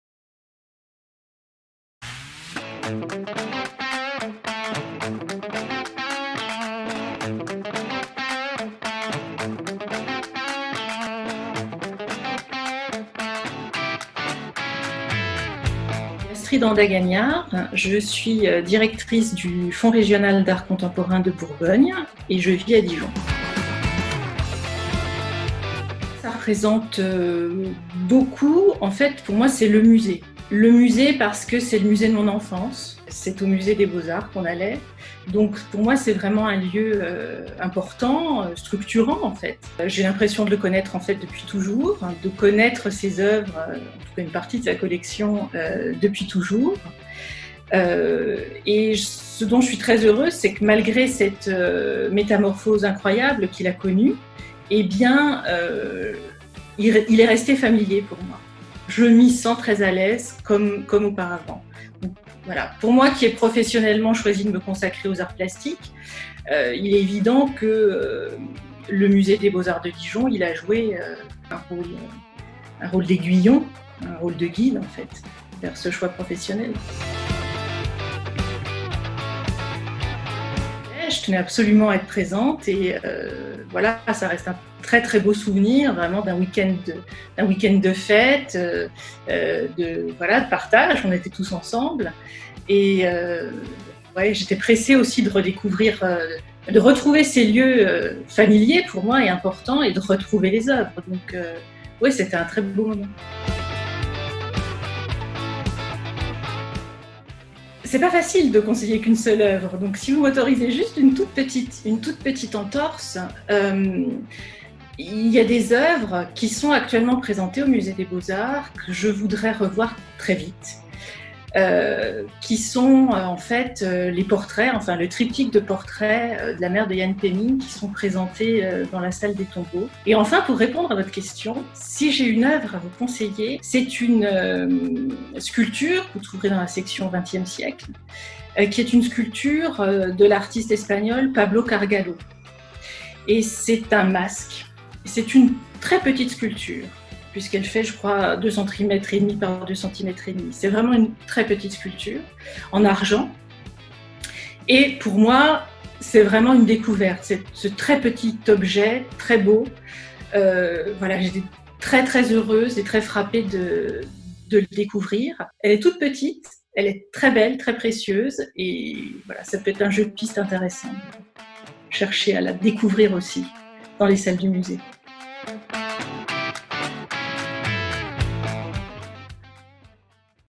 1 an du musée des Beaux-Arts de Dijon : Astrid Handa-Gagnard
Dijon1anMBA | Astrid Handa-Gagnard raconte "son" MBA
Il y a un an, le musée des Beaux-Arts de Dijon, métamorphosé après 10 ans de travaux, rouvrait ses portes. Depuis, vous êtes plus de 300 000 à l'avoir visité, à avoir redécouvert ses oeuvres restaurées, à vous l'être approprié encore et encore, comme Astrid Handa-Gagnard, directrice du FRAC de Bourgogne.